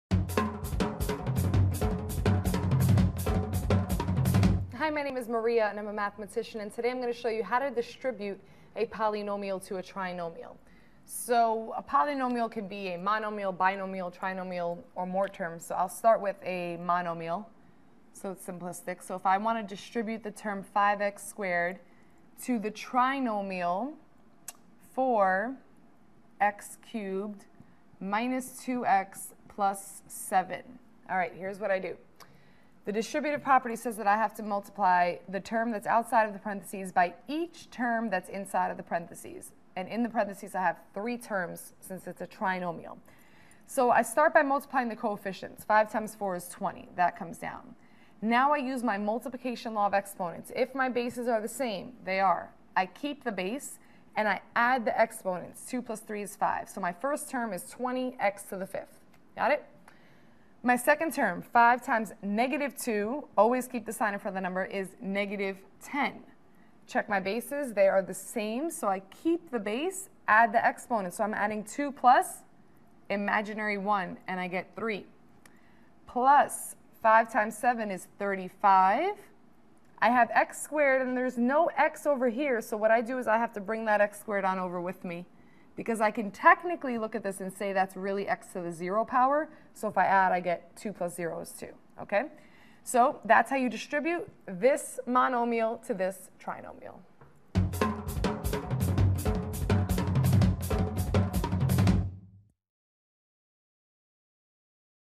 How to Distribute a Polynomial to a Trinomial
How to Distribute a Polynomial to a Trinomial. Part of the series: Algebra Help. Distributing a polynomial to a trinomial is something that must be done in a very specific way. Distribute a polynomial to a trinomial with help from a mathematician in this free video clip.